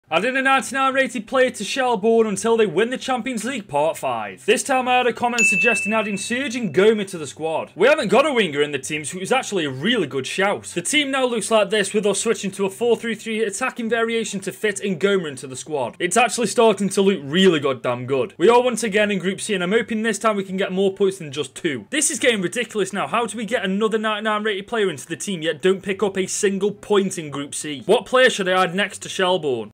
Adding a 99 Rated Player To Shellbourne Until We Win The Champions League (Part 5)
If you enjoyed those.. you may enjoy these!